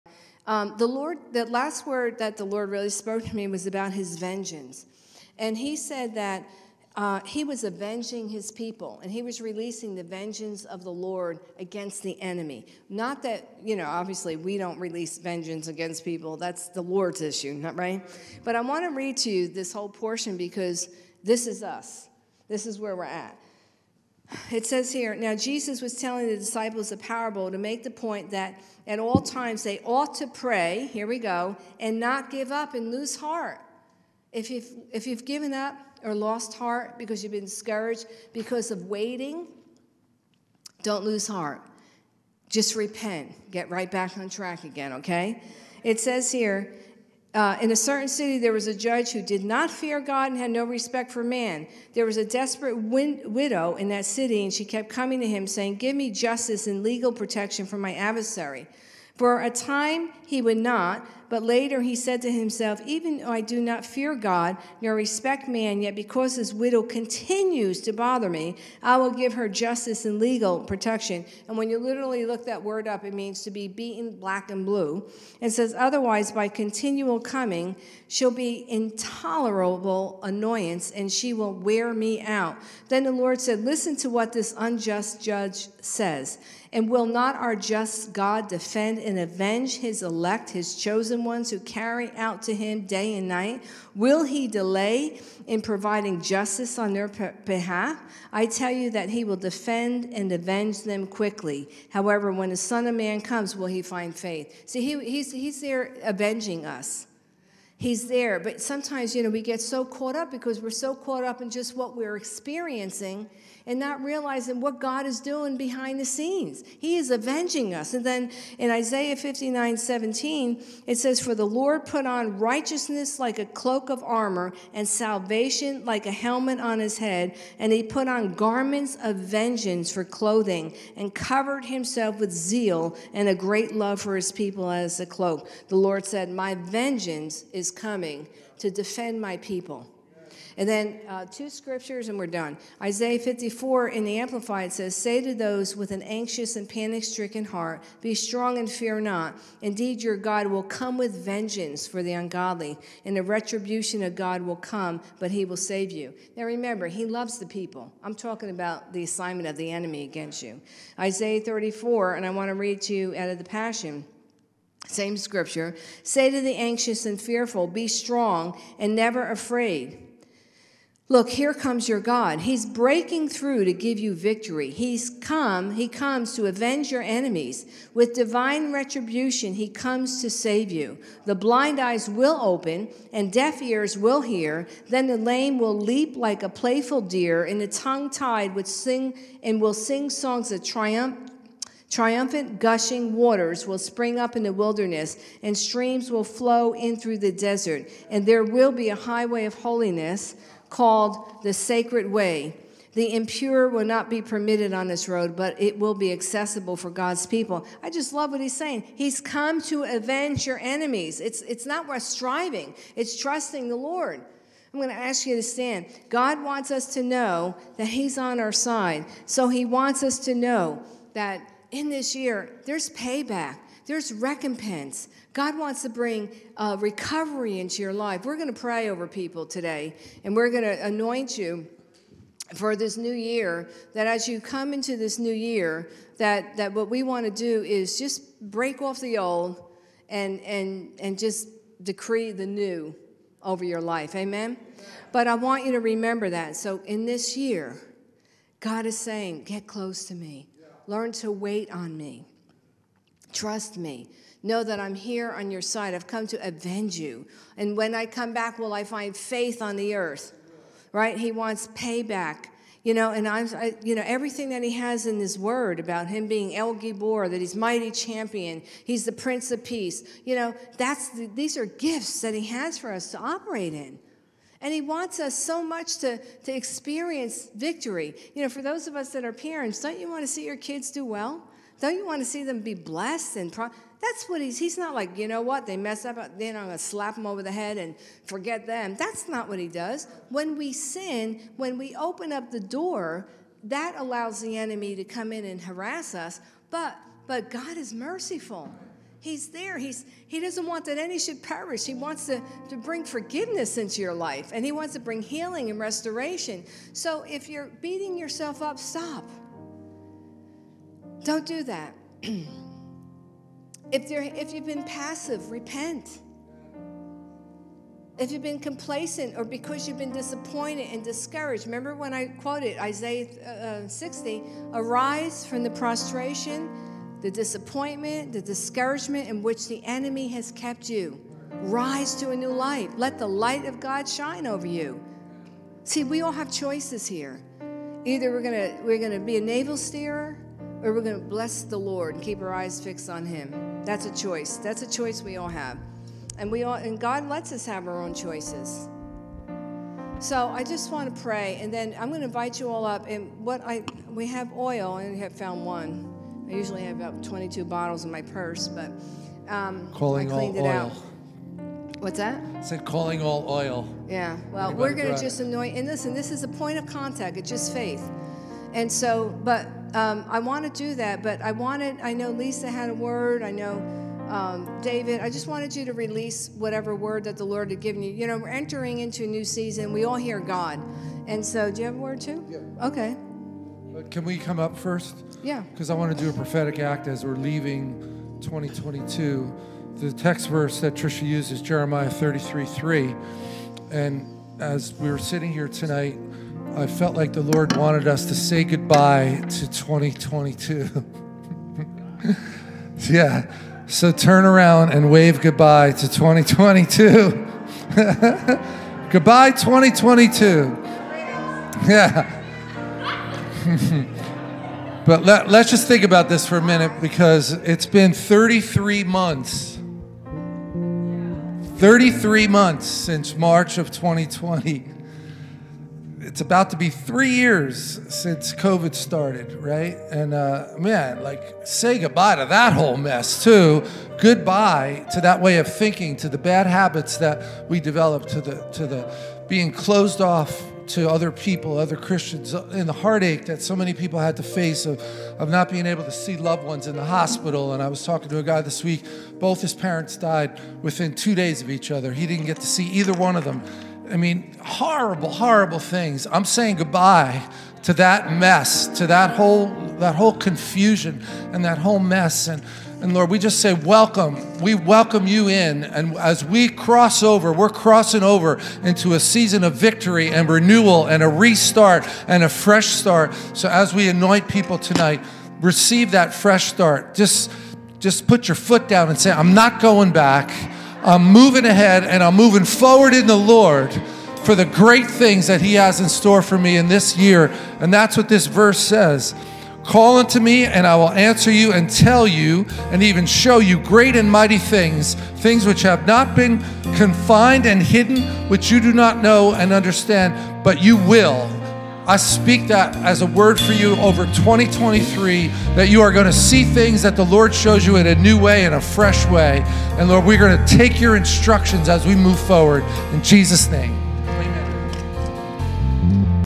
Trisha Roselle: When the Son of Man Comes Will He Find Faith? (Luke 18:8)
Trisha speaks at King of Kings Worship Center on 12/28/2022.  Quotes: this year, God is saying, get close to me. Learn to wait on me. Trust me, know that I'm here on your side, I've come to avenge you, and when I come back will I find faith on the Earth? He wants payback, and everything that he has in this word about him being El Gibbor, that he's Mighty Champion, he's the Prince of Peace, these are gifts that he has for us to operate in. And he wants us so much to experience victory.

The Lord, the last word that the Lord really spoke to me
was about his vengeance. He said that he was
avenging his people, and he was releasing
the vengeance of the Lord against the enemy.
Not that, obviously we don't release vengeance
against people, that's the Lord's issue,
but I want to read to you this whole portion,
because this is us, this is where we're at.
It says here, now Jesus was telling the disciples
a parable to make the point that at all times
they ought to pray, here we go, and not give up and lose heart.
If you've given up or lost heart because you've been
discouraged because of waiting, don't lose heart.
Just repent, get right back on track again.
It says here, in a certain city there was a judge
who did not fear God and had no respect for man.
There was a desperate widow in that city
and she kept coming to him saying, give me justice
and legal protection from my adversary.
For a time he would not, but later he said to himself,
even though I do not fear God, nor respect man,
yet because this widow continues to bother me
I will give her justice and legal protection,
and when you literally look that word up it means
to be beaten black and blue,
and it says, otherwise by continual coming
she'll be intolerable annoyance and she will wear me out.
Then the Lord said, listen to what this unjust judge says,
and will not our just God defend and avenge his elect,
his chosen ones, who carry out to him day and night?
Will he delay in providing justice on their behalf?
I tell you that he will defend and avenge them quickly,
however when the son of man comes will he find faith?
See he's there avenging us.
He's there, but sometimes we get so caught up,
because we're so caught up in just what we're experiencing
and not realizing what God is doing behind the scenes.
He is avenging us. Then in Isiah 59:17, it says for the Lord
put on righteousness like a cloak of armor
and salvation like a helmet on his head,
and he put on garments of vengeance for clothing,
and covered himself with zeal, and a great love
for his people as a cloak.
The Lord said, my vengeance is coming to defend my people.
And then two scriptures and we're done.
Isiah 54 in the Amplified it says, say to those
with an anxious and panic stricken heart,
be strong and fear not, indeed your God
will come with vengeance for the ungodly,
and a retribution of God will come, but he will save you.
Now remember, he loves the people.
I'm talking about the assignment of the enemy against you.
Isiah 34, and I want to read to you out of the Passion,
same scripture, say to the anxious and fearful
be strong and never afraid.
Look, here comes your God, he's breaking through
to give you victory.
He comes to avenge your enemies with divine retribution,
he comes to save you.
The blind eyes will open, and deaf ears will hear,
then the lame will leap like a playful deer,
and the tongue tied will sing songs of triumph,
triumphant gushing waters will spring up in the wilderness
and streams will flow in through the desert,
and there will be a highway of holiness called The Sacred Way.
The impure will not be permitted on this road,
but it will be accessible for God's people.
I just love what he's saying.
He's come to avenge your enemies.
It's not we're striving, it's trusting the Lord.
I'm going to ask you to stand.
God wants us to know that he's on our side,
so he wants us to know that in this year
there's payback, there recompense,
God wants to bring recovery into your life.
We're going to pray over people today,
and we're going to anoint you for this new year
that as you come into this new year
that what we want to do is just break off the old,
and just decree the new over your life, amen.
But I want you to remember that.
And so in this year, God is saying, get close to me.
Learn to wait on me.
Trust me, know that I'm here on your side,
I've come to avenge you, and when I come back
will I find faith on the Earth?